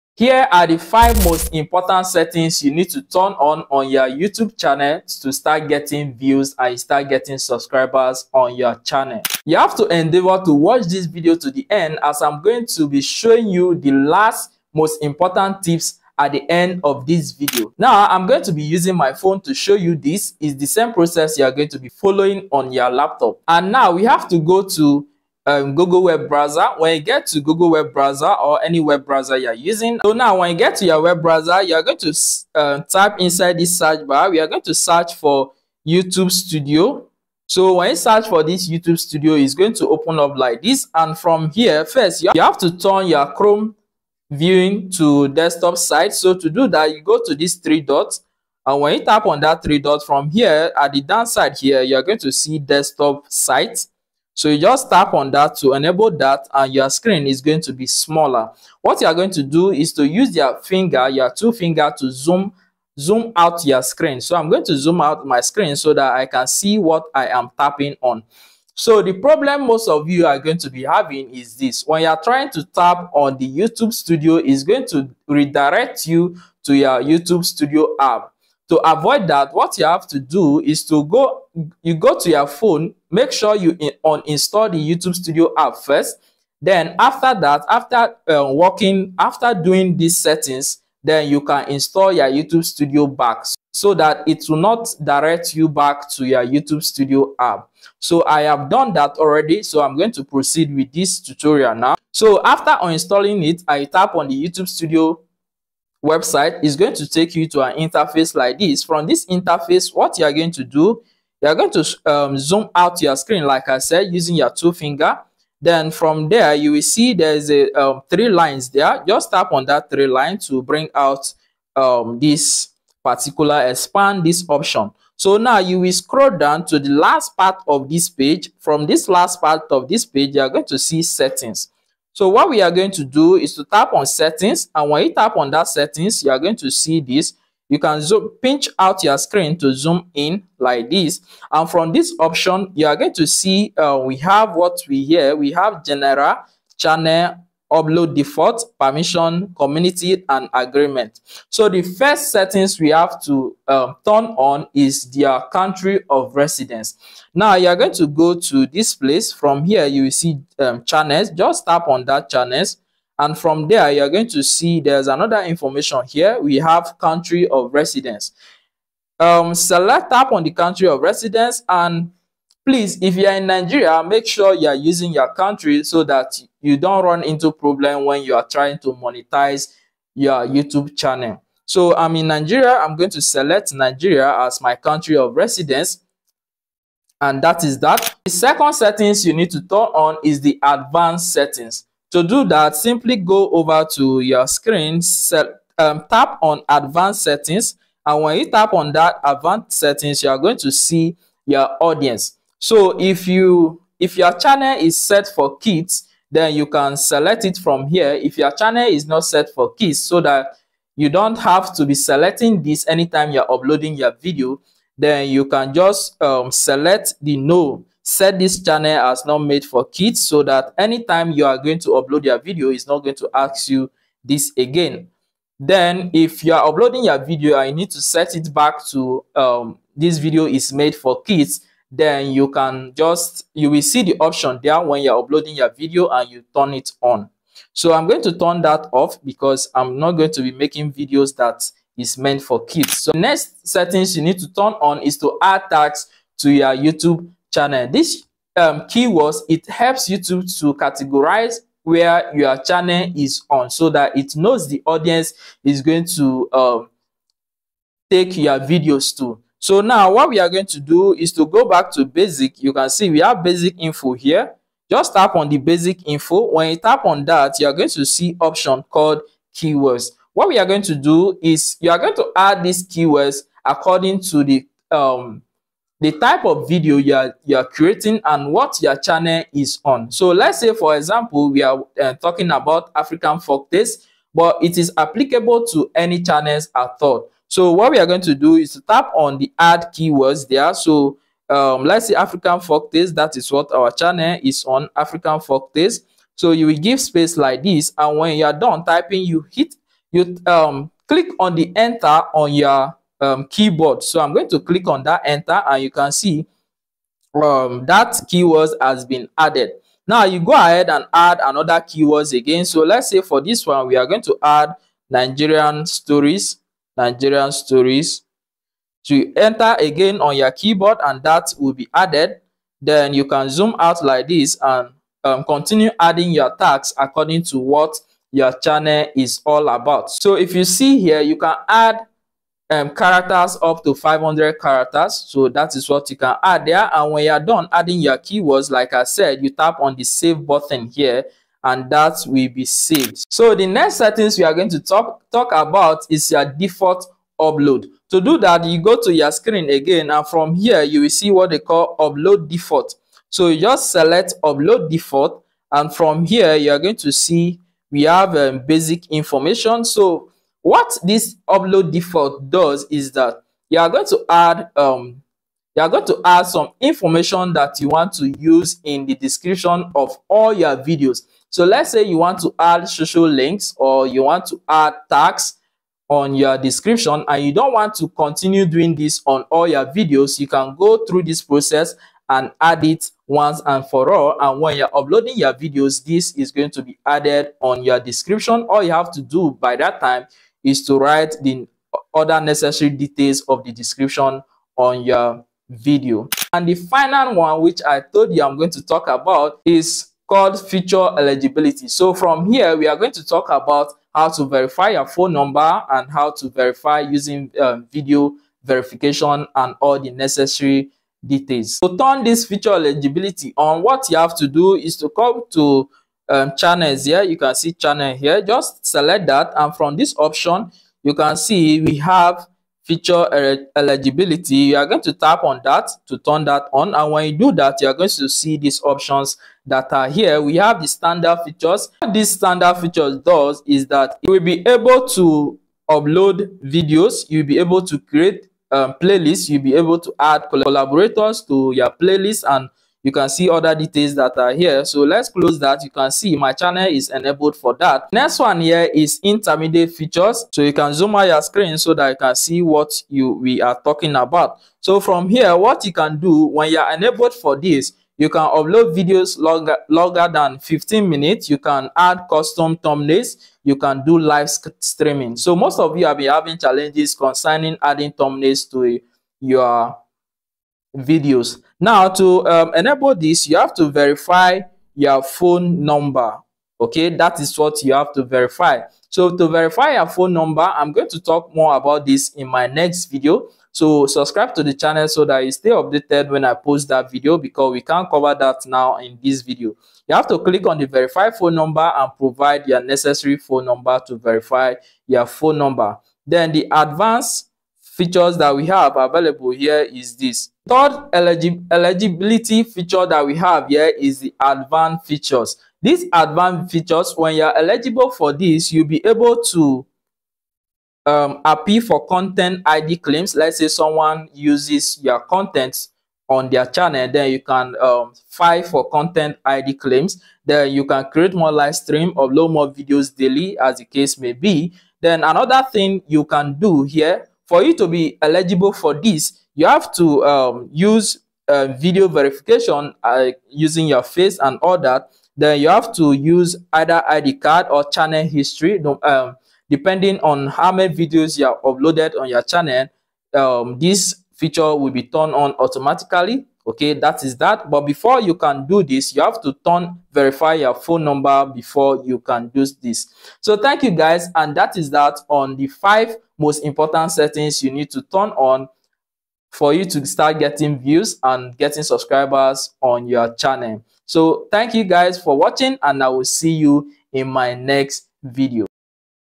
CHANGE this SETTINGS on Your YOUTUBE CHANNEL to get MORE VIEWS
CHANGE the SETTINGS on YOUTUBE to get MORE VIEWS in 2024

In this video I will show you the five most advanced youtube settings to get more views on youtbe,  then we will also talk about how to get more views on youtube 5 secret settings, and important settings to get more views on youtube.